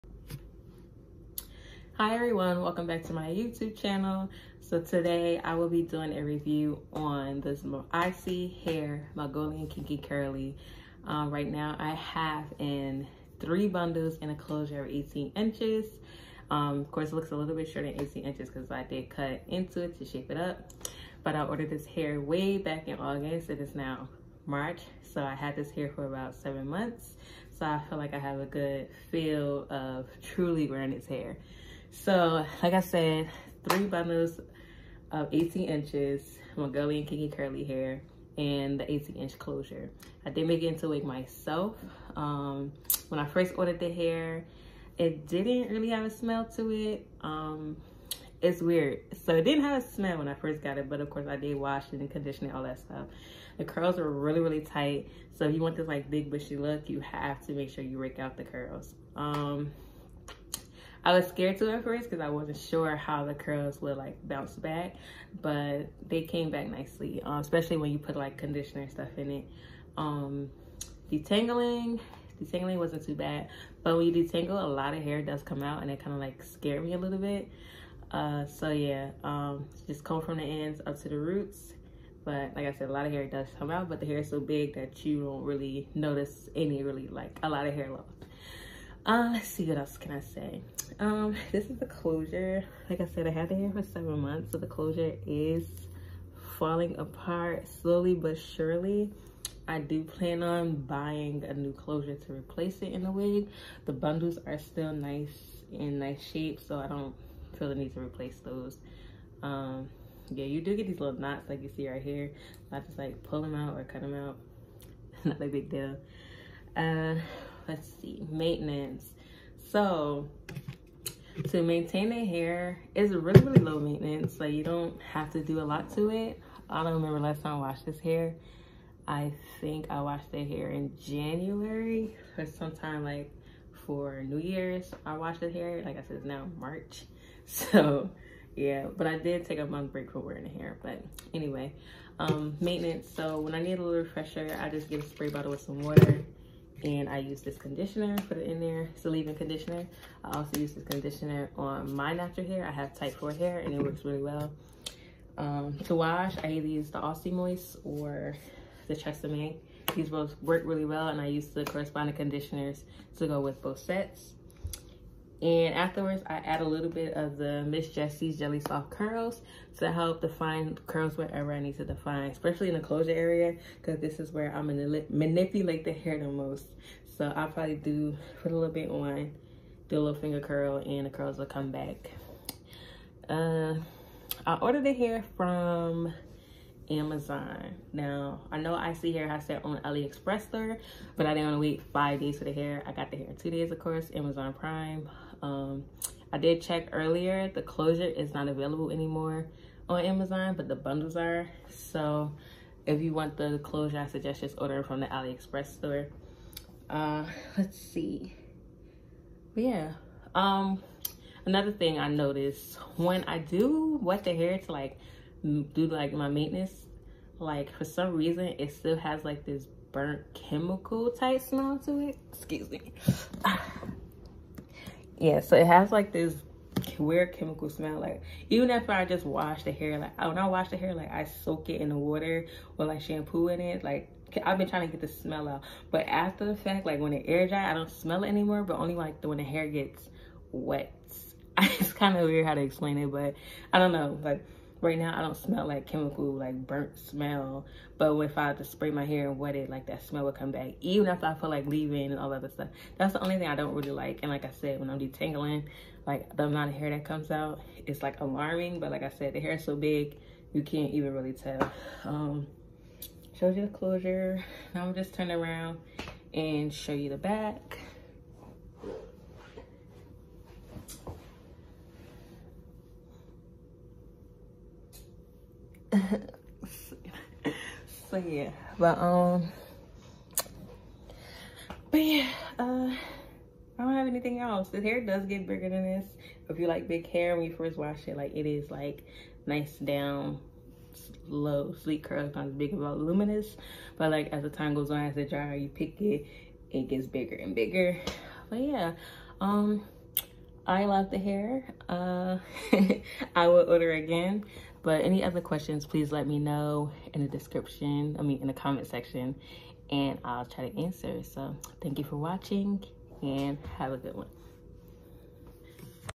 ISee Hair-Mongolian Kinky Curly (7 Month Review)
Hey guys!! In the video I am wearing 3 bundles of 18 inches with an 18 inch closure of mongolian kinky curly hair.